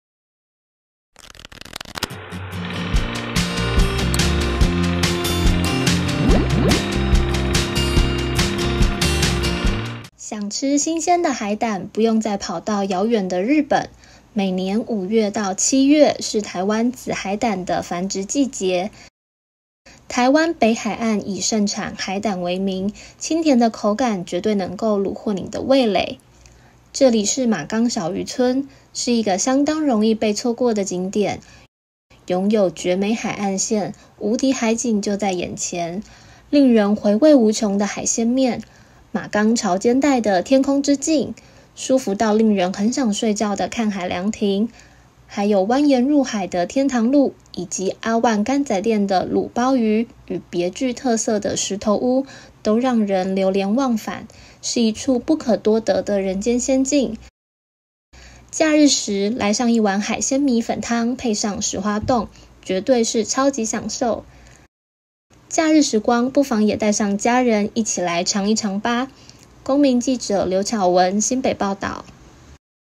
基隆觀光｜海鮮美食｜全台最東邊秘境漁港 紫海膽馬崗登場
(中嘉基隆新聞 公民記者劉巧雯/貢寮區採訪報導)
每年5-6月是台灣紫海膽盛行的季節，來到台灣最右邊的小漁村-馬崗漁港就能嘗到新鮮的海味，還有幾隻伸懶腰的貓咪躲在樹叢下，假日的午後多了遊人來到馬崗，找貓、聽海與喝一杯極東咖啡，嫻靜又愜意的生活令許多人相當嚮往。